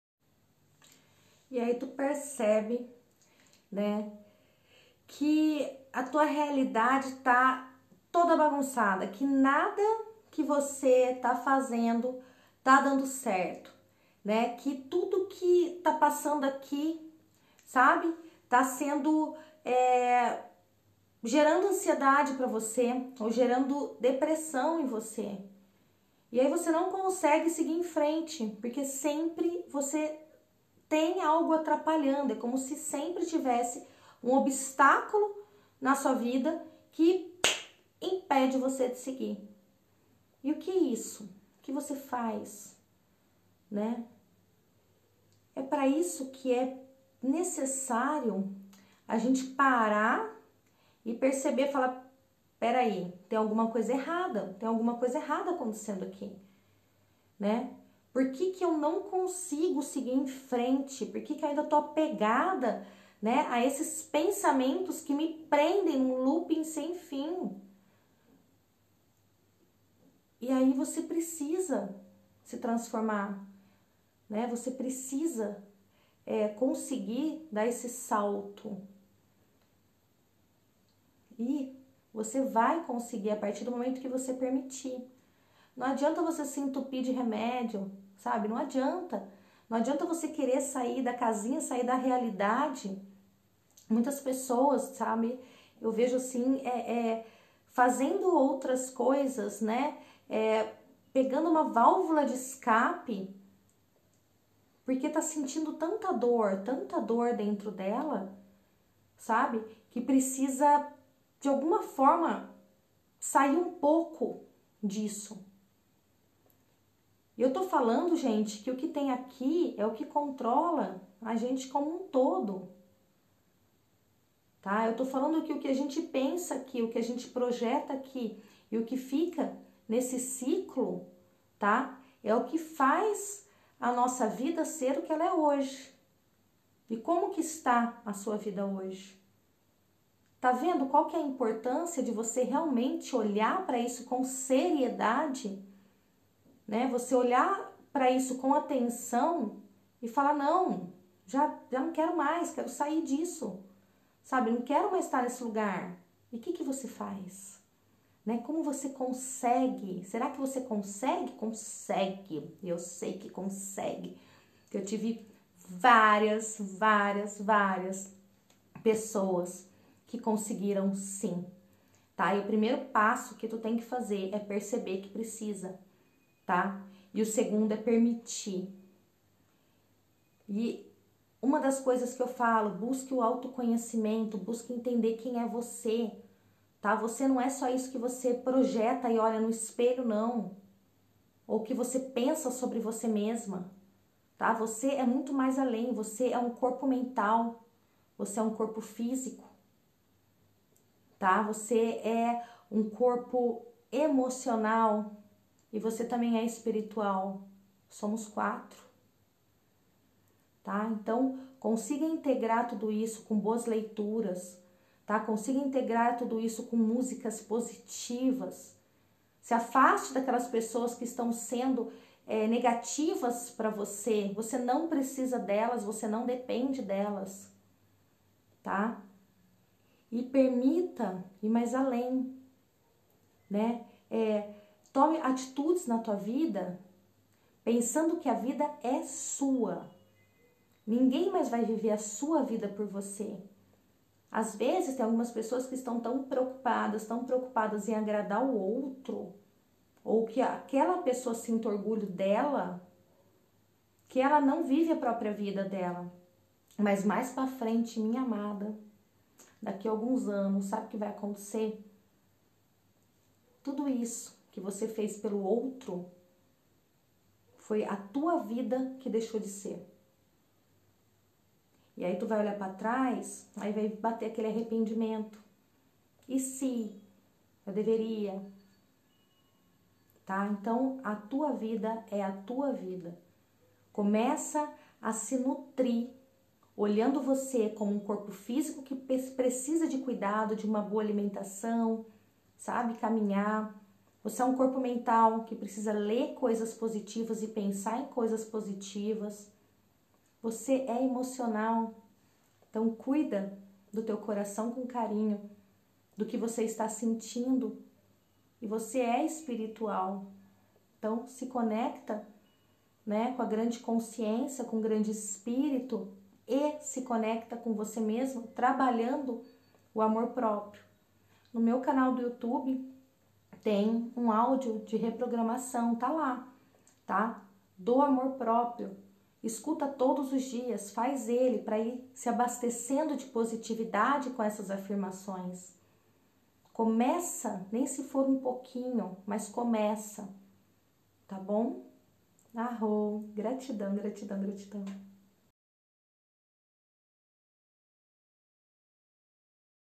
Minha vida não anda, não vai pra frente. O que está acontecendo? | Vida Desperta
Sempre quando algo parece dar certo,
volta pra trás?

O que está acontecendo? Como posso mudar isso?

Assiste o vídeo que dou dicas de como pode melhorar a sua vida e para você refletir o que pode estar ocorrendo😉

Aproveita e já clica no link abaixo e vem participar da Turma de Agosto do VIDA DESPERTA e já muda essa realidade! 👇

Aho! Mitakuye Oyasin 🙌